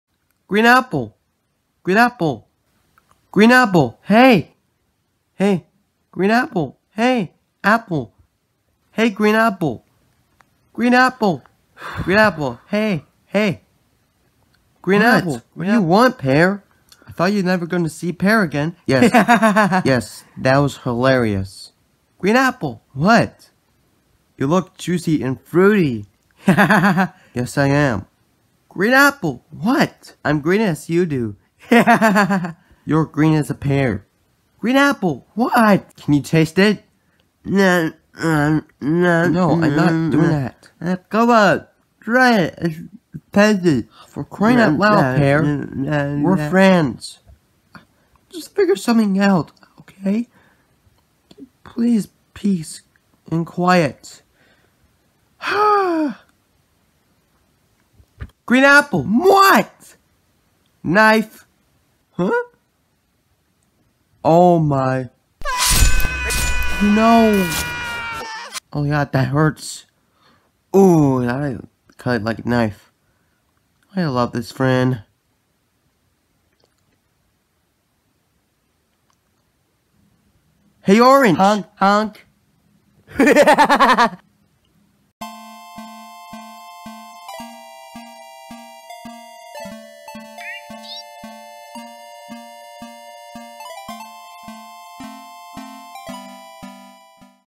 SuperPear5 (Original)
Pear and Green apple were friends, and his other friend is Funny Orange.